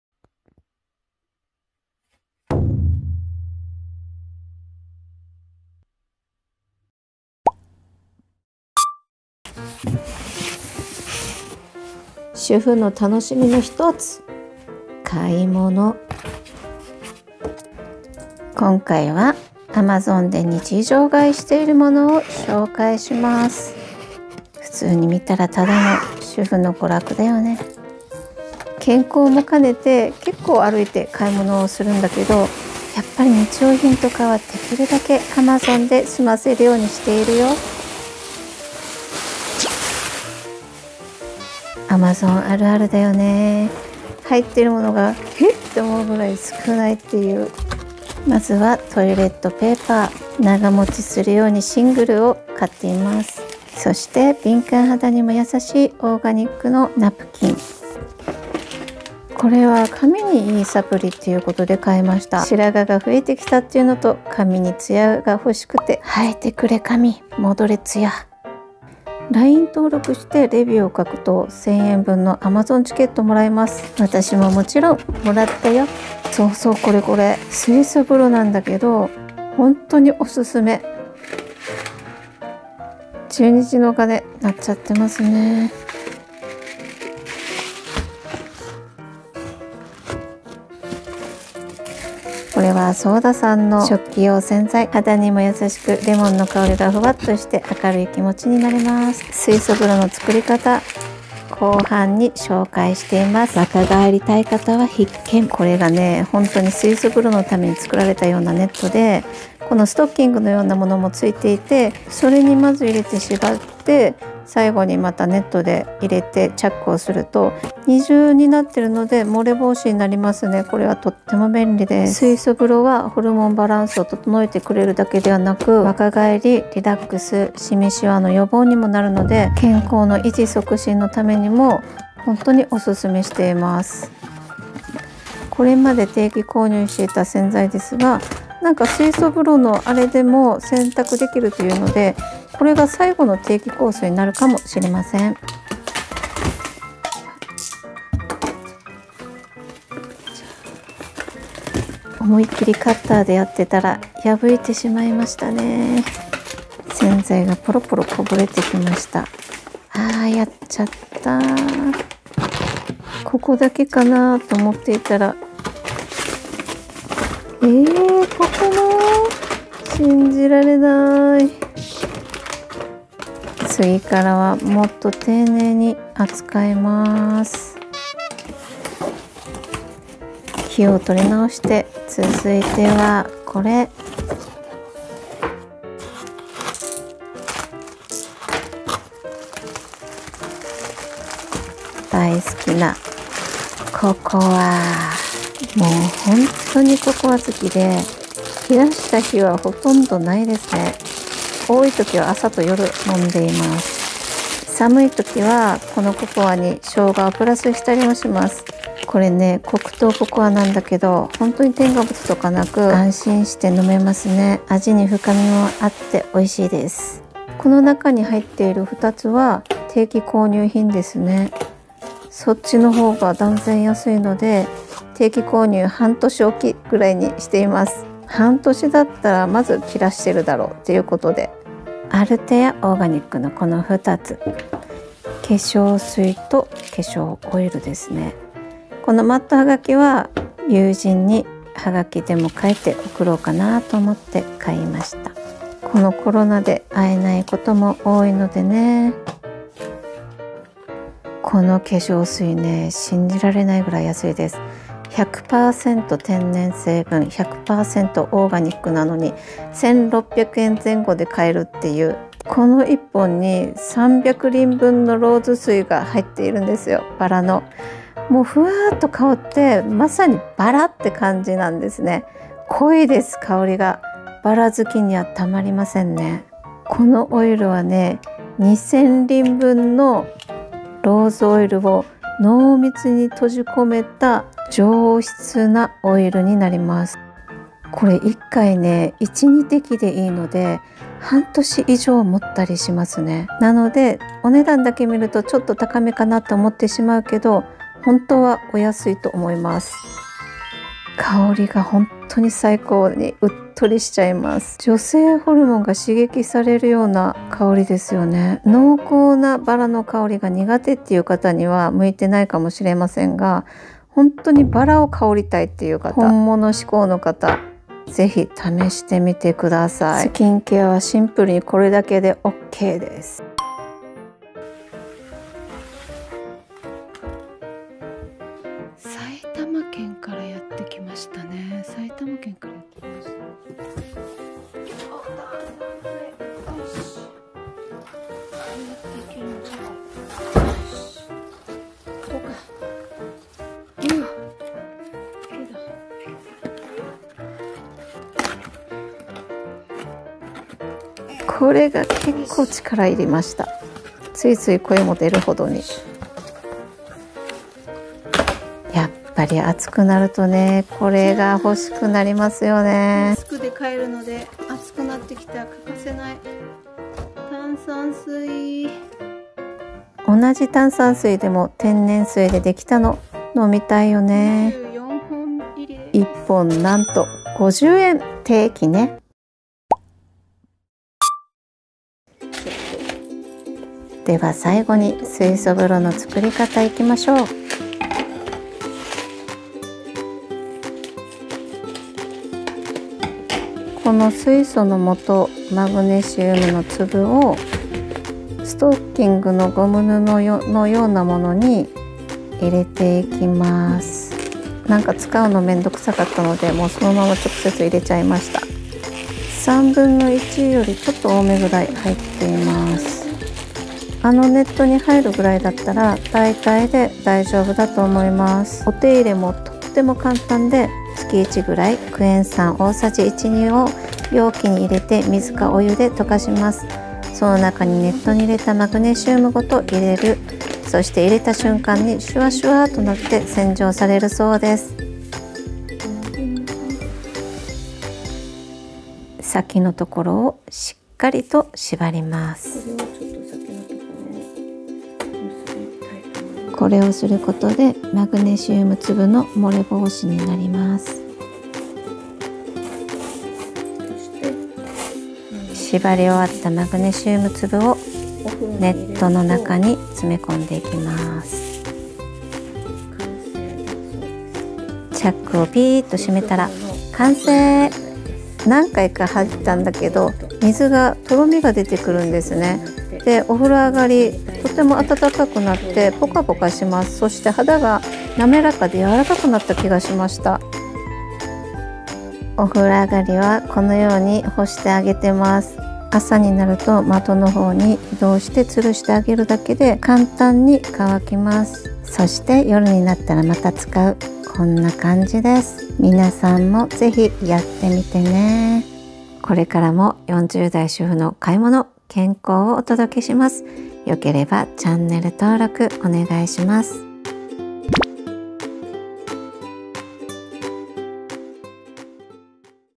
【アマゾン購入品】40代主婦の大満足すぎる購入品/日常そんなに買うの？/若返る！水素風呂作り方
今回の動画は40代主婦がアマゾンで大量購入した買い物を紹介しています。後半に若返り水素風呂の作り方も。参考にして健康維持促進のお役に立てれば嬉しいです♪︎

これからもおば活をたのしく更新していきます。

毎日の健康·買い物·些細な変化をたのしむおば活

撮影機材
Galaxynote10+